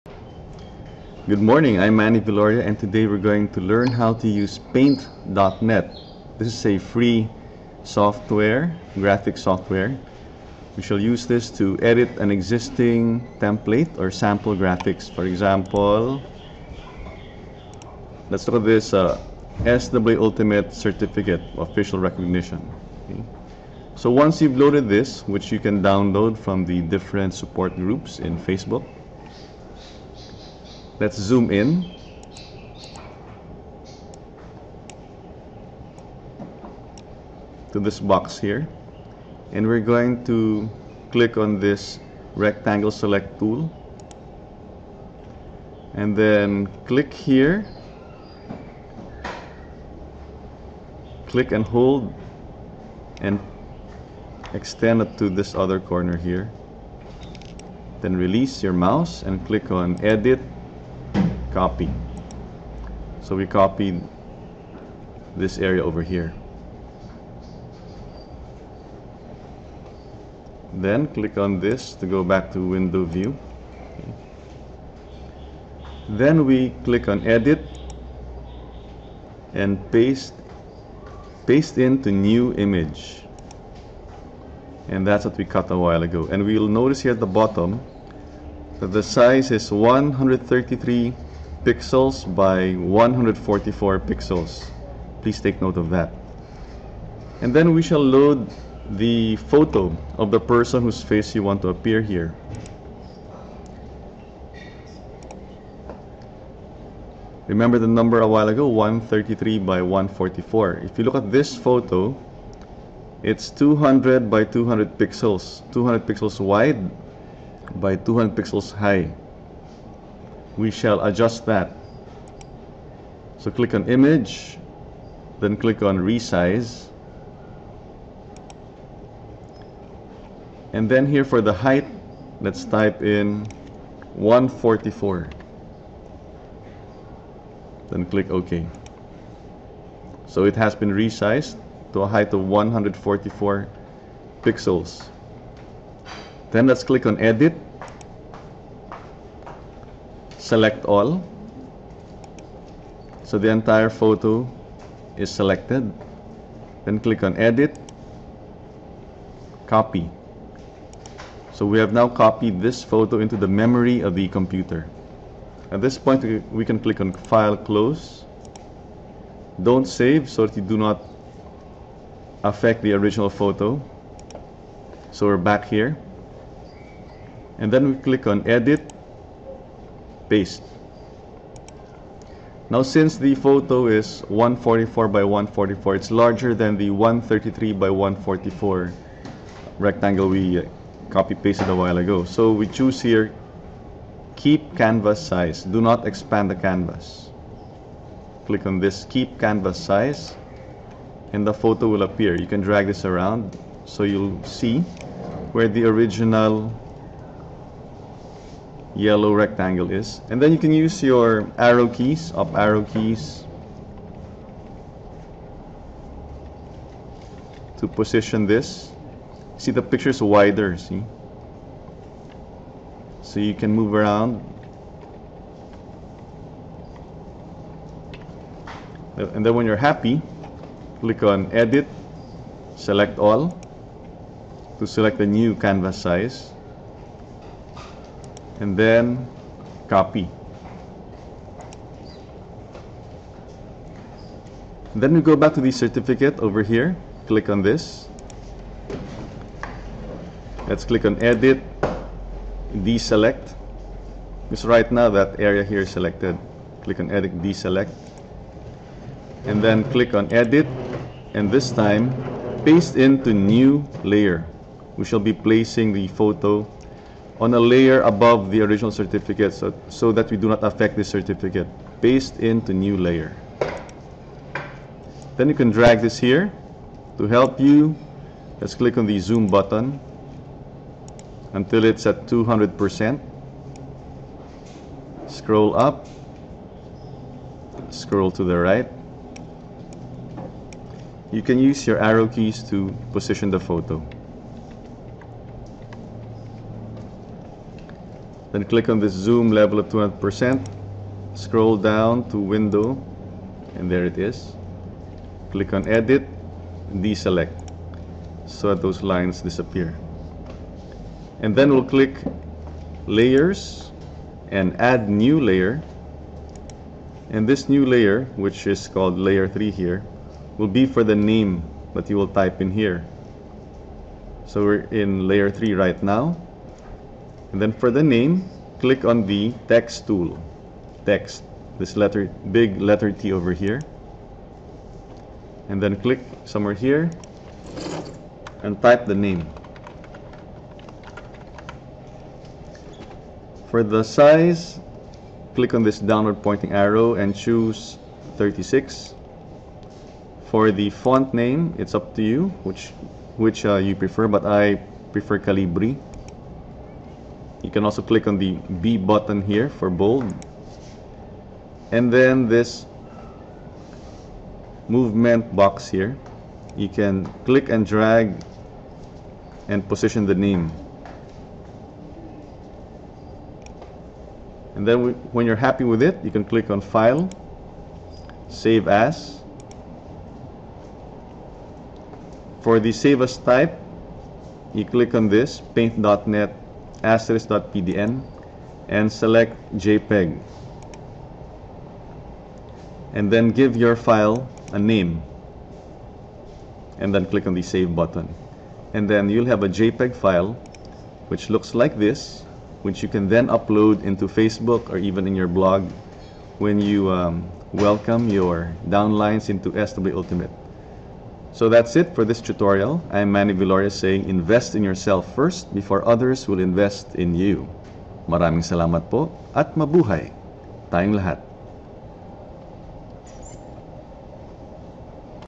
How To Edit SWA Ultimate Certificates Using Paint.net, by Manny Viloria
Manny Viloria demonstrates how to edit a SWA Ultimate Certificate of Recognition using Paint.net (a free graphics software).